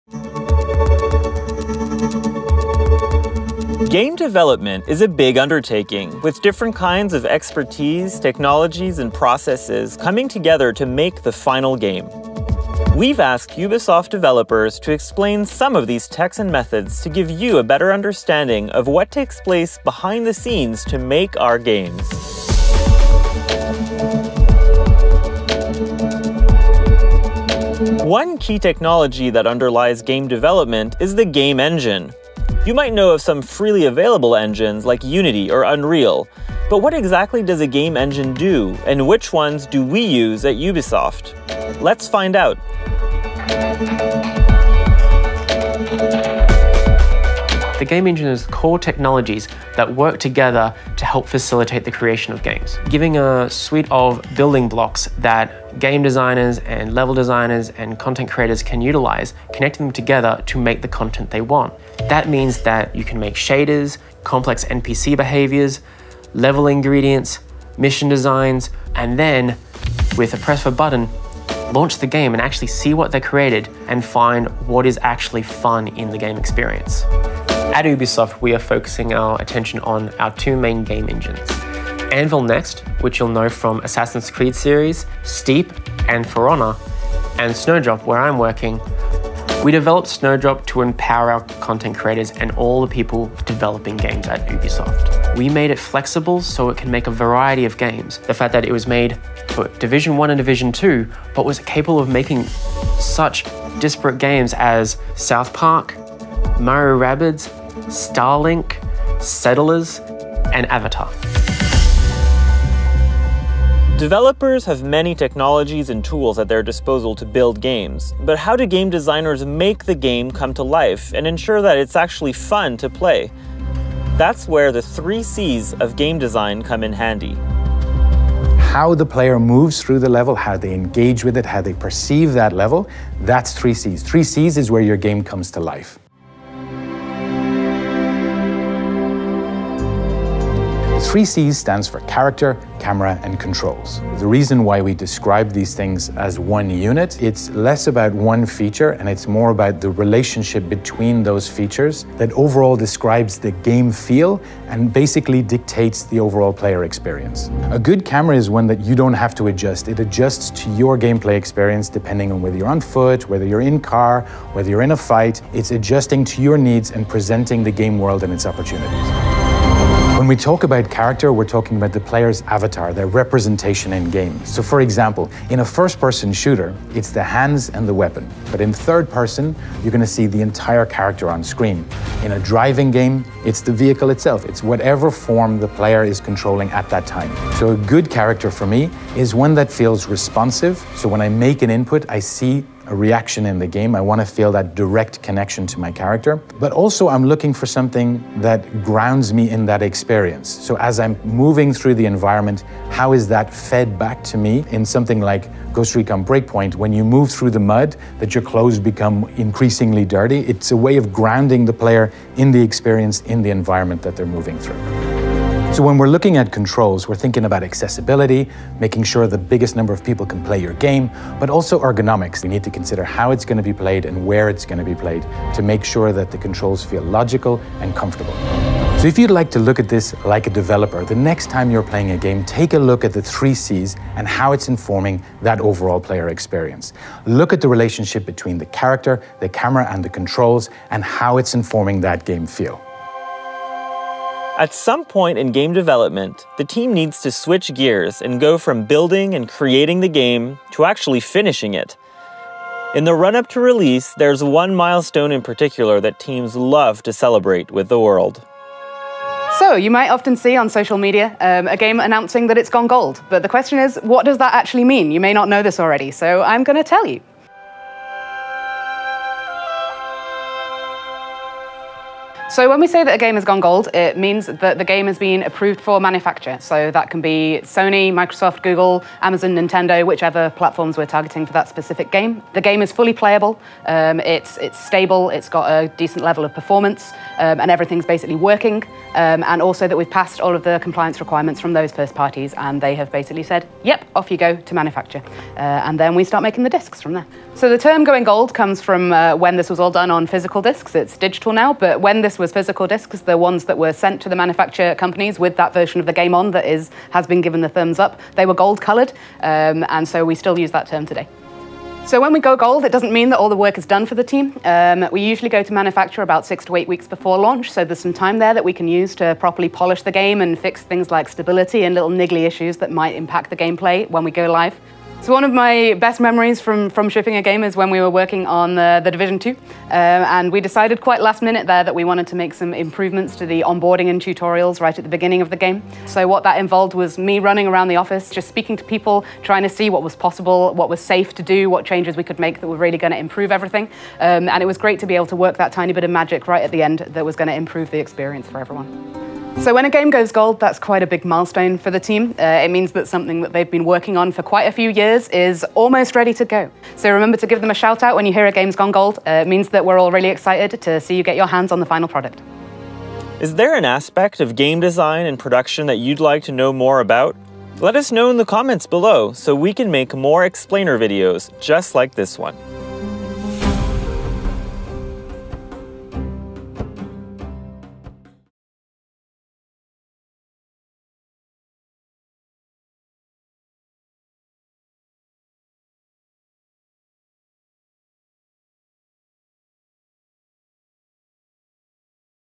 Games Explained: From Game Engines to Going Gold
Ubisoft Devs share a glimpse of the technologies and processes they regularly utilize to bring games to life: game engines, the 3 Cs of game design, and the approval process to release a game.